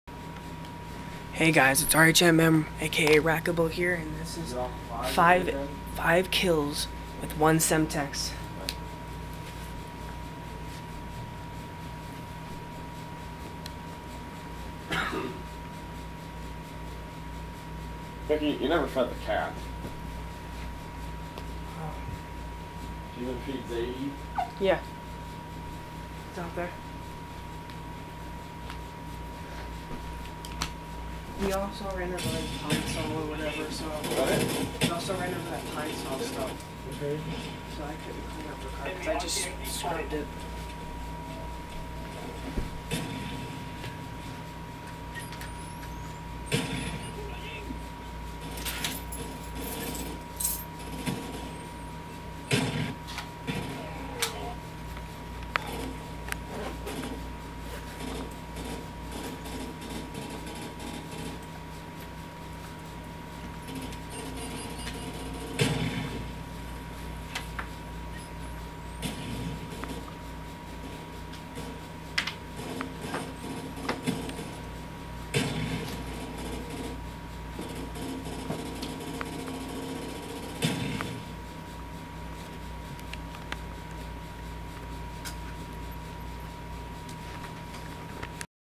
5 kills 1 semtex ((amazing))
the game just started and i went up the hill and tossed a semtex WITH OUT even thinking about it and i got a killstreak of 5. but that killstreak ends just after i through it because a guy killed me right after i though it (but still amazing) hope you like ;p